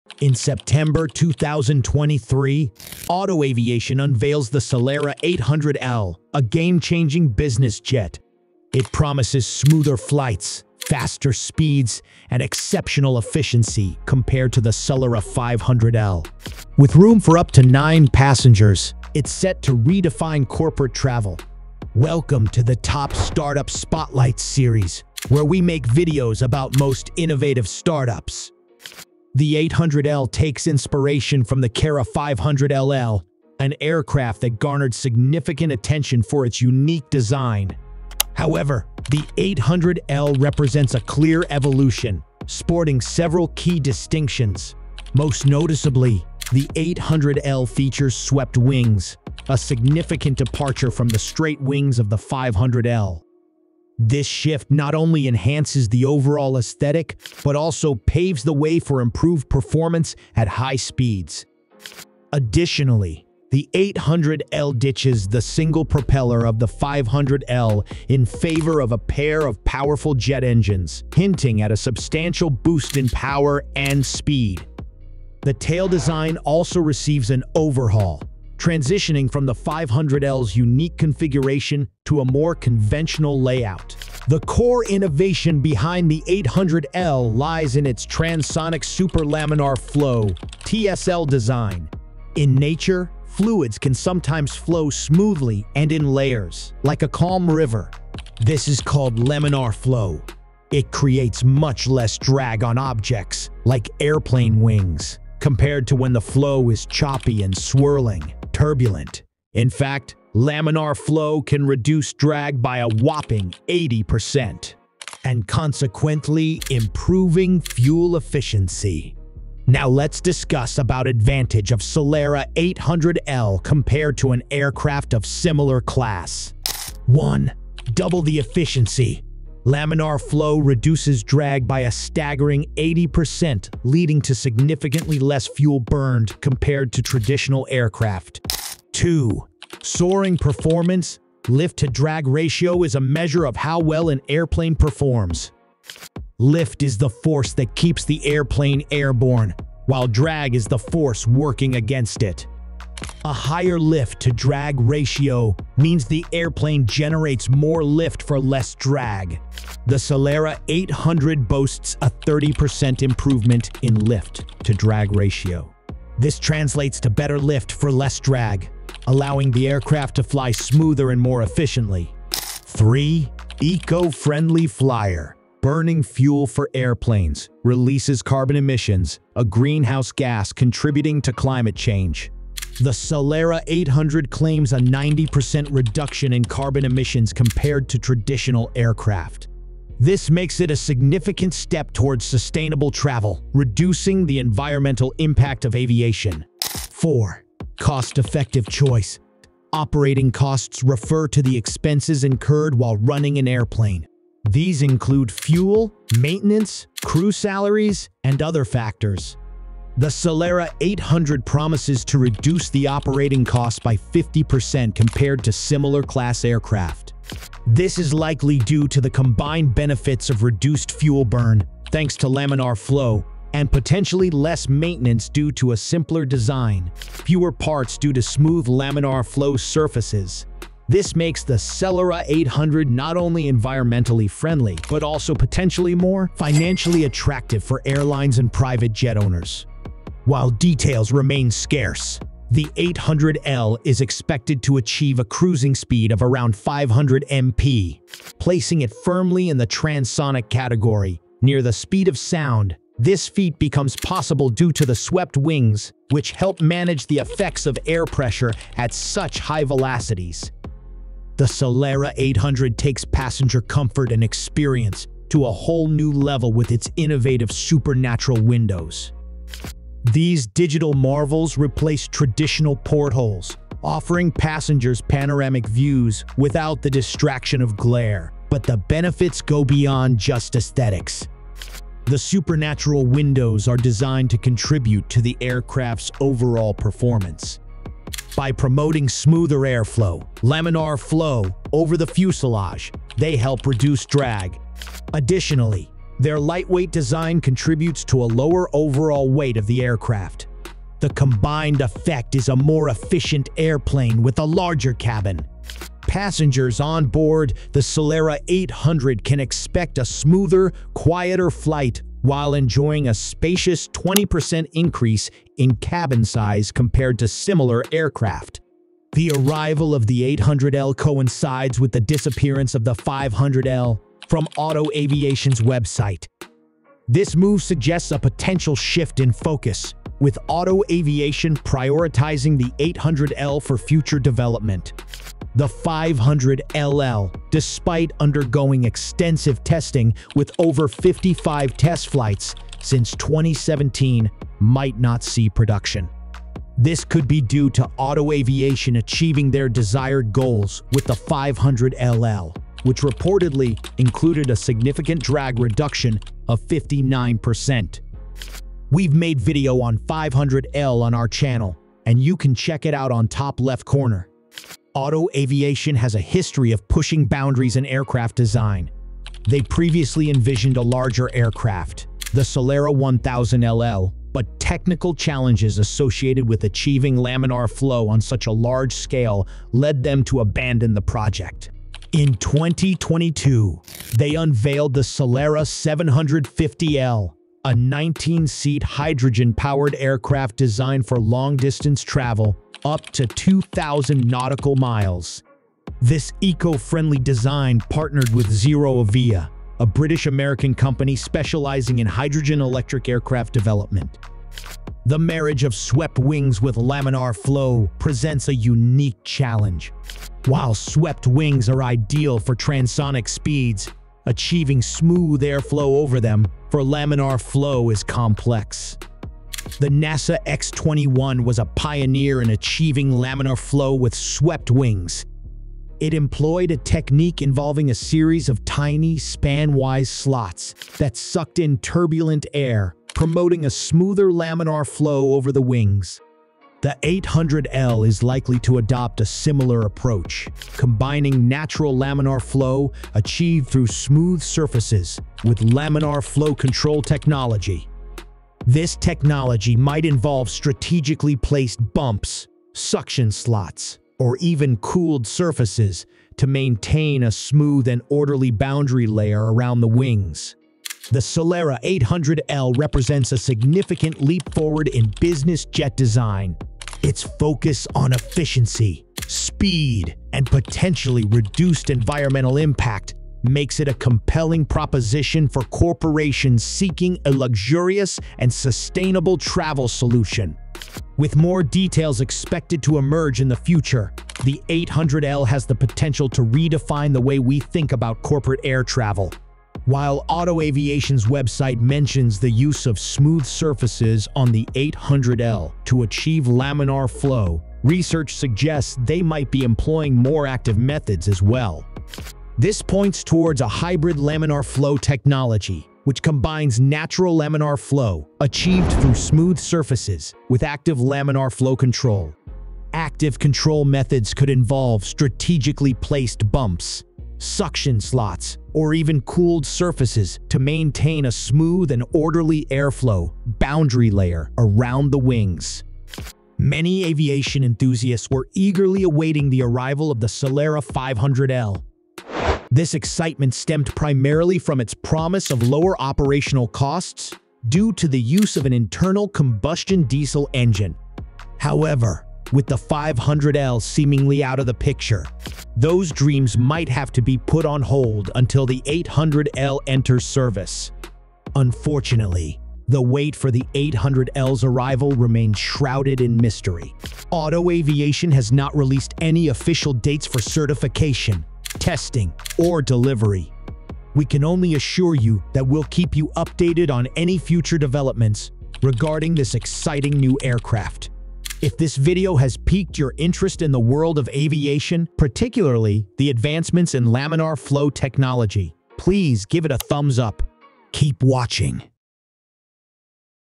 Otto Celera 800L: Modern Evolution of the Celera 500L – Fastest, Most Efficient Aircraft Innovation
In September 2023, Auto Aviation unveils the Celera 800L, a game-changing business jet. It promises smoother flights, faster speeds, and exceptional efficiency compared to the Celera 500L. With room for up to nine passengers, it's set to redefine corporate travel. Welcome to the Top Startup Spotlights Series where we make videos about most innovative startups. The 800L takes inspiration from the Celera 500L, an aircraft that garnered significant attention for its unique design. However, the 800L represents a clear evolution, sporting several key distinctions. Most noticeably, the 800L features swept wings, a significant departure from the straight wings of the 500L. This shift not only enhances the overall aesthetic but also paves the way for improved performance at high speeds. Additionally, the 800L ditches the single propeller of the 500L in favor of a pair of powerful jet engines, hinting at a substantial boost in power and speed. The tail design also receives an overhaul, transitioning from the 500L's unique configuration to a more conventional layout.
The core innovation behind the 800L lies in its transonic super laminar flow (TSL) design. In nature, fluids can sometimes flow smoothly and in layers, like a calm river. This is called laminar flow. It creates much less drag on objects, like airplane wings, compared to when the flow is choppy and swirling (turbulent). In fact, laminar flow can reduce drag by a whopping 80%... and consequently improving fuel efficiency.

What about Otto Celera 800: When compared to an aircraft of a similar class: 30% enhancement in lift-to-drag ratio
60% reduction in fuel burn
90% decrease in carbon emissions
 Half the operating cost

The Celera 800L represents a significant leap forward in business jet design. Its focus on efficiency, speed, and potentially reduced environmental impact makes it a compelling proposition for corporations seeking a luxurious and sustainable travel solution. With more details expected to emerge in the future, the 800L has the potential to redefine the way we think about corporate air travel.

★★★★★ You can also watch the Following Related Videos on Top Startup Spotlights ★★★★★